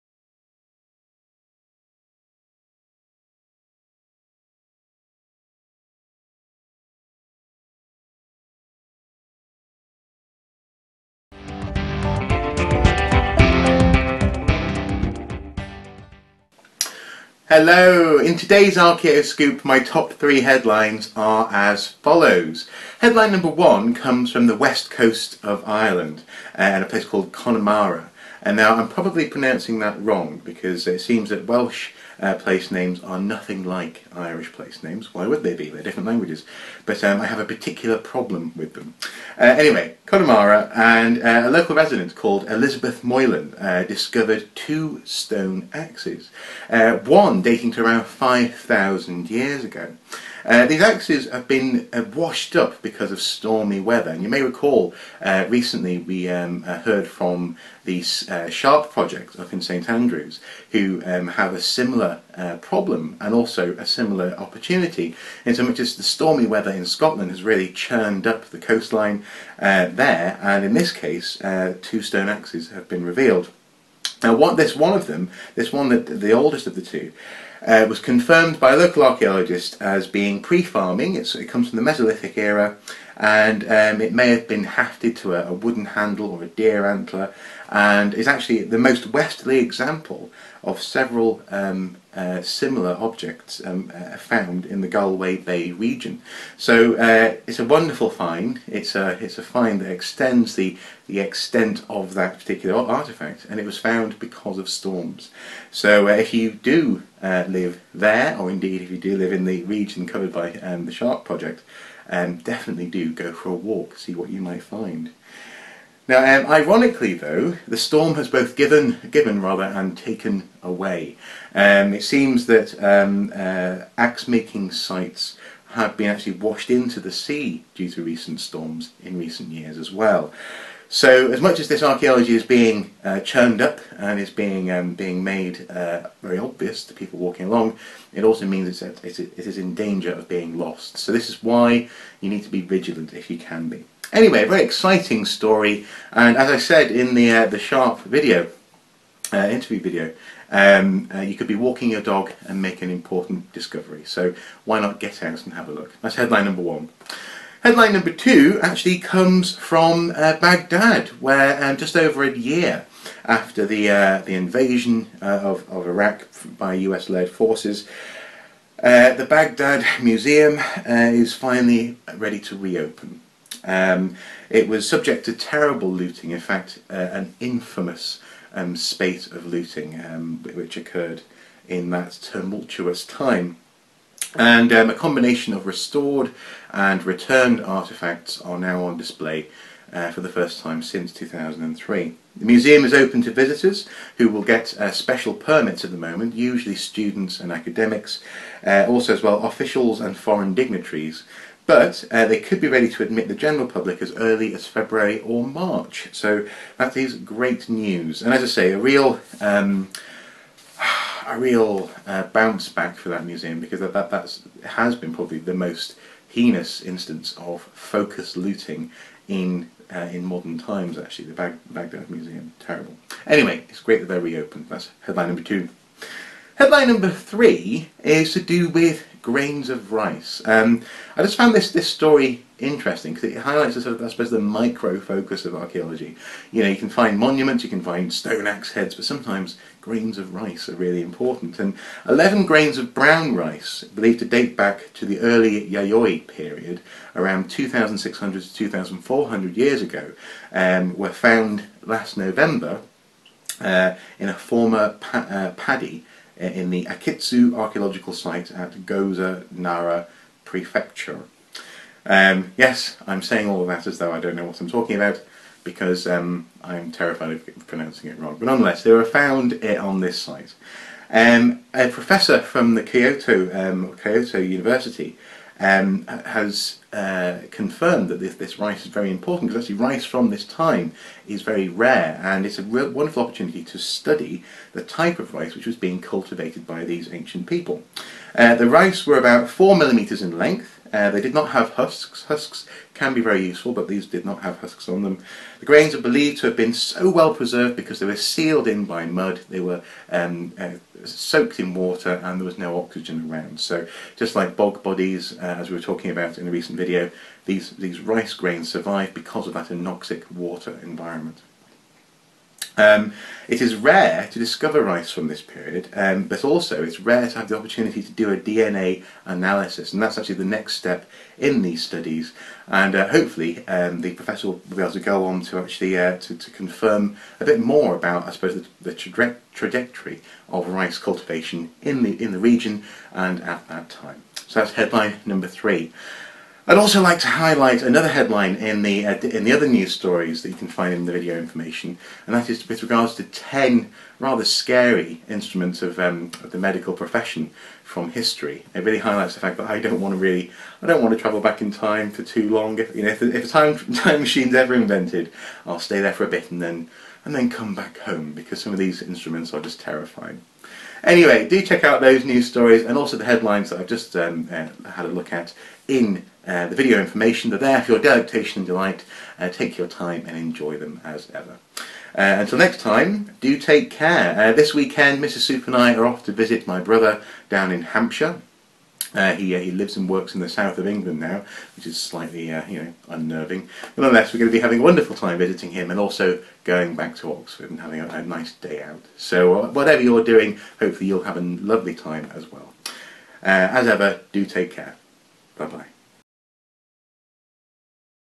Archaeoscoop: Storms Reveal Mesolithic Axe in Ireland
Welcome to Archaeoscoop, the place to find heritage and archaeology related news stories from around the world!

Today, axes are washed up on the coast of Ireland, the Baghdad Museum reopens and rice is recovered...